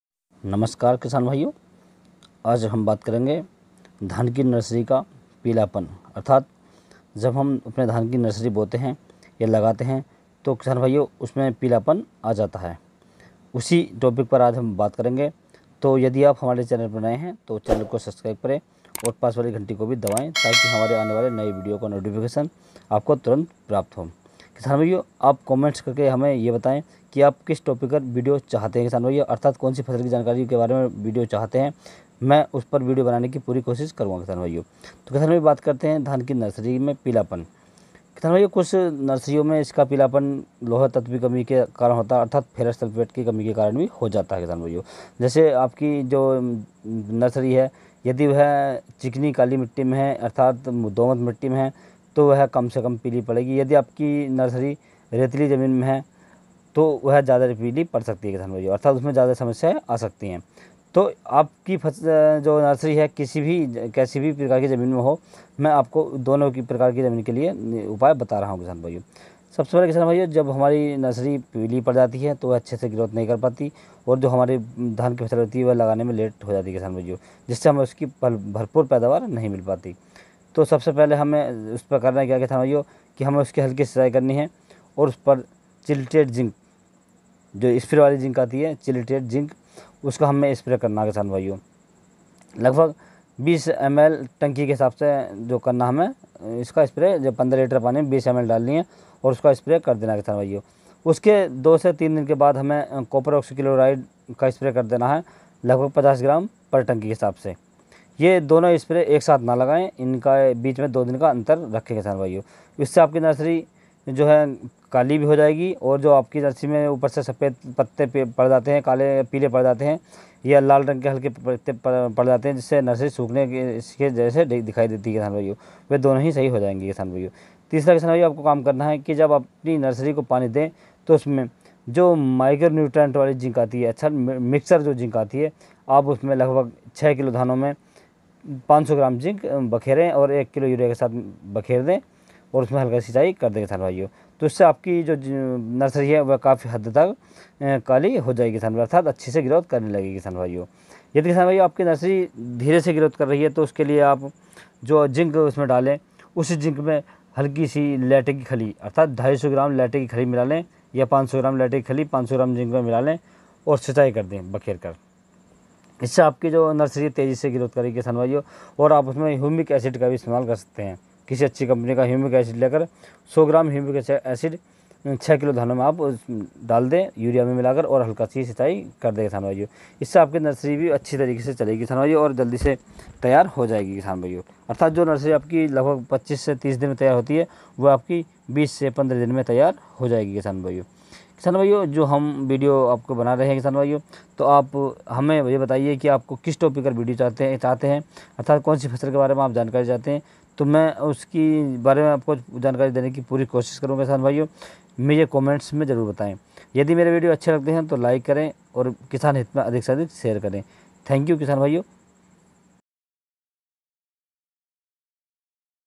धान की नर्सरी में पीलापन दूर करने का आसान तरीका/dhan ki nursery mein pilapan kaise dur karen
नमस्कार किसान भाइयों आज की इस वीडियो में मैंने आपको धान की नर्सरी में पीलापन तथा उसकी रुकी हुई ग्रोथ को कैसे तेजी से बढ़ाएं इस बारे में आपको संपूर्ण जानकारी दी है यदि मेरे द्वारा दी गई जानकारी आपको अच्छी लगी है तो वीडियो को अधिक से अधिक किसान हित में शेयर करें धन्यवाद

Thanks